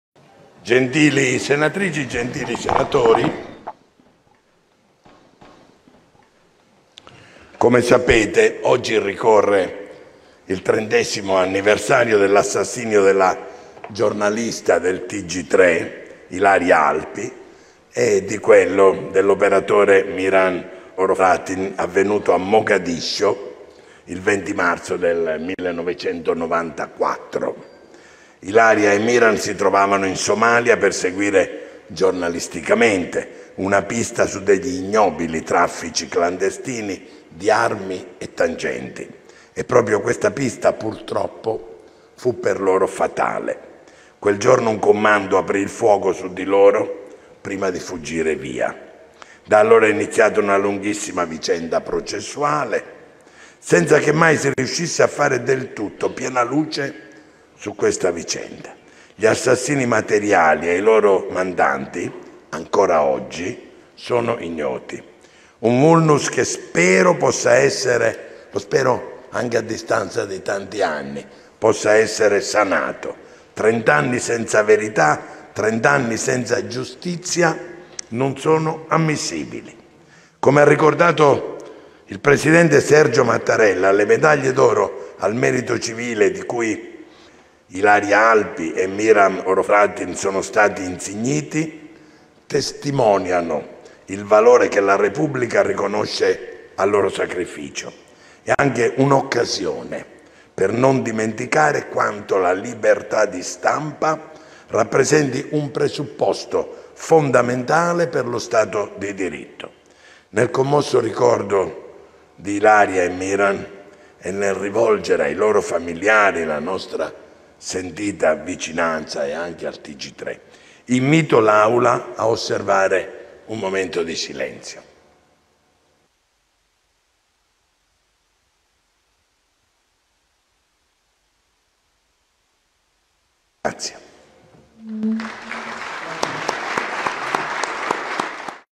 IL PRESIDENTE LA RUSSA RICORDA ILARIA ALPI E MIRAN HROVATIN IN SENATO
(Agenzia Italia News) IL PRESIDENTE LA RUSSA RICORDA ILARIA ALPI E MIRAN HROVATIN IN SENATO